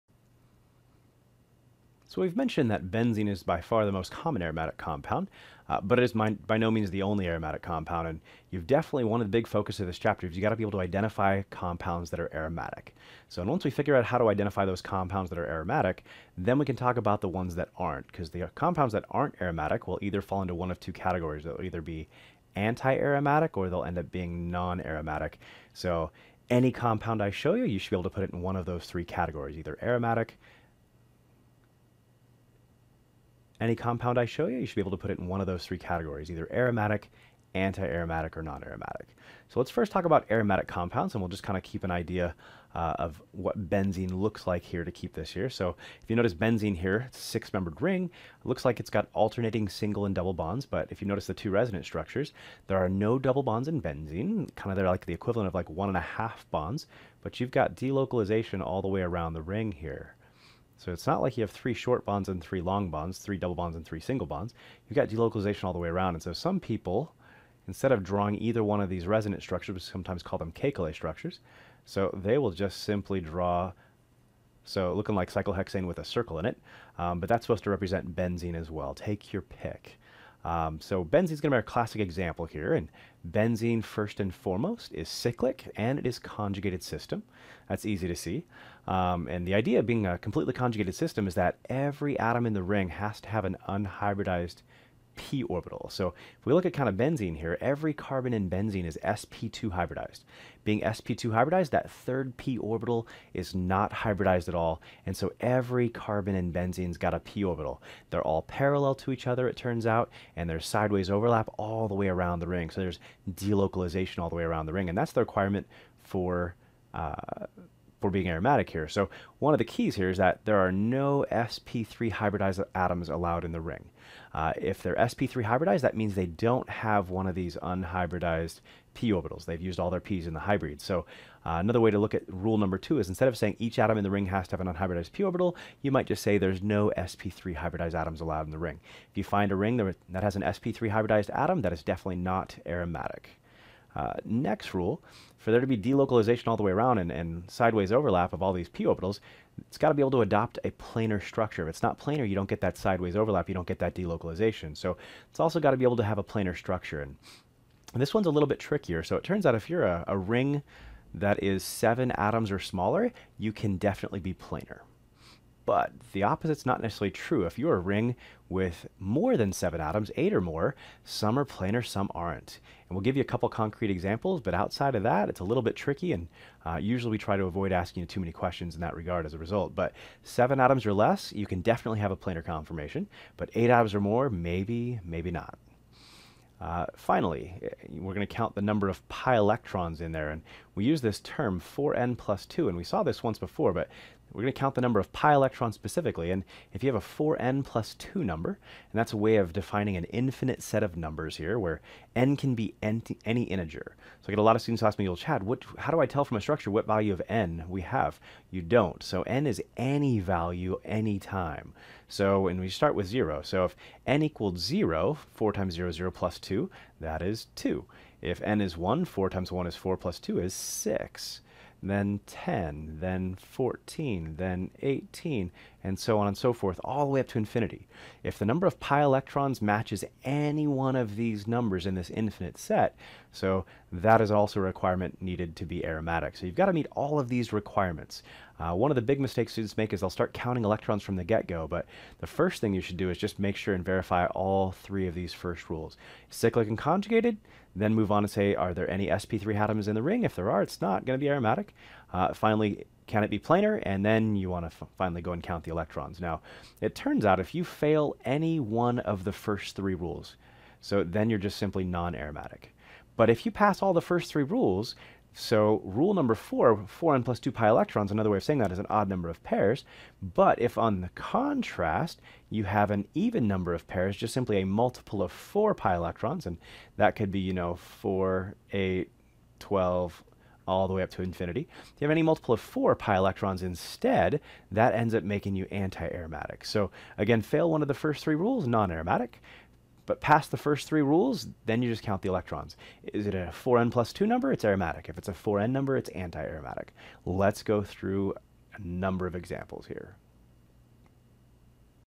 17.2a Criteria for Aromatic Compounds
Chad breaks down the criteria that make a compound Aromatic and explains the difference that makes a compound Nonaromatic or Antiaromatic.

Happy Studying!